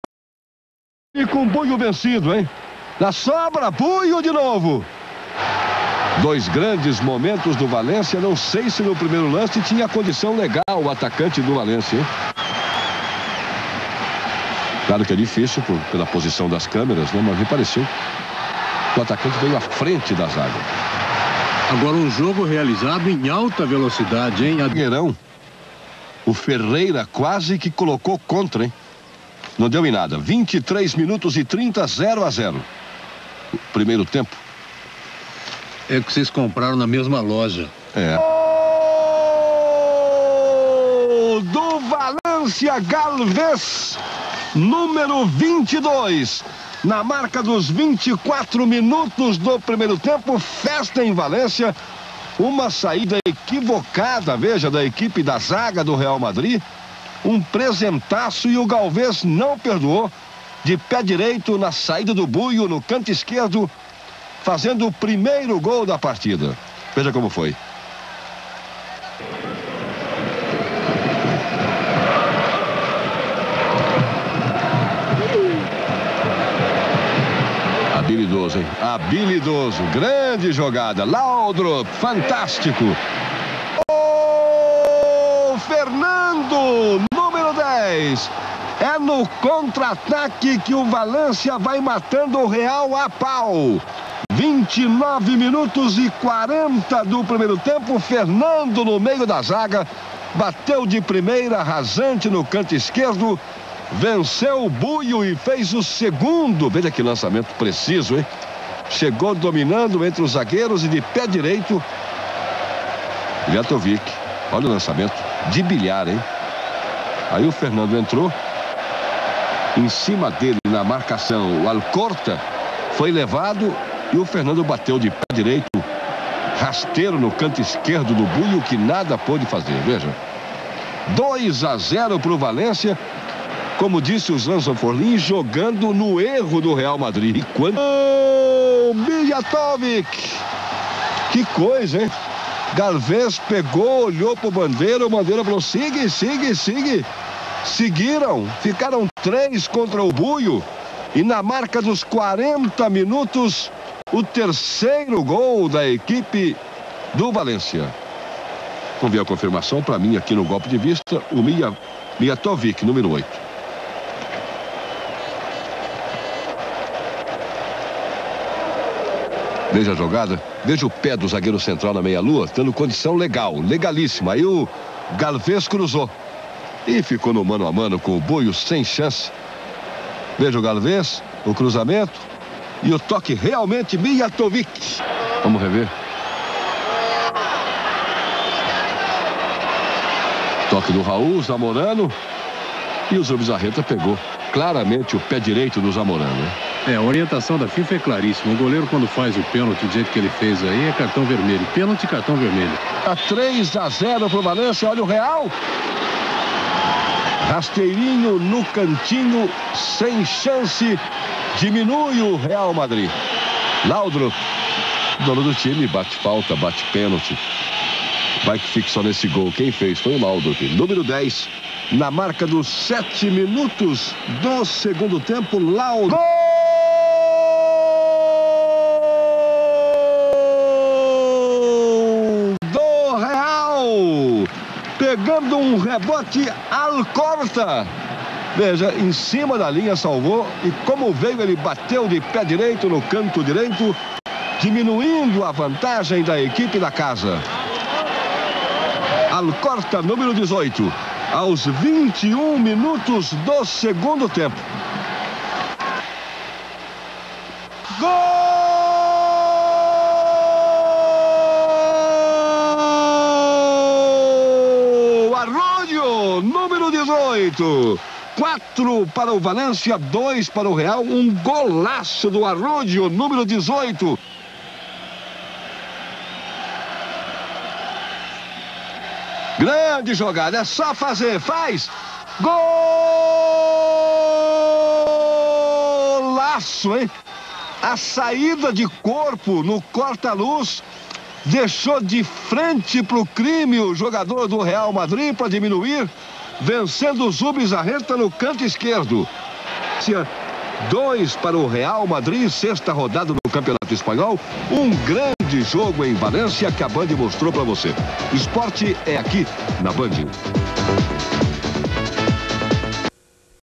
VALENCIA 4x3 REAL MADRID - Campeonato Espanhol 1995/96 (Gols) - Band
Data: 03/10/1995
Local: Estádio de Mestalla - Valencia - Espanha
Gols: Galvez(V)/Fernando(V)/Mijatovic(V)/Michael Laudrup(RM)/Alkorta(RM)/Arroyo(V)/Michel(RM)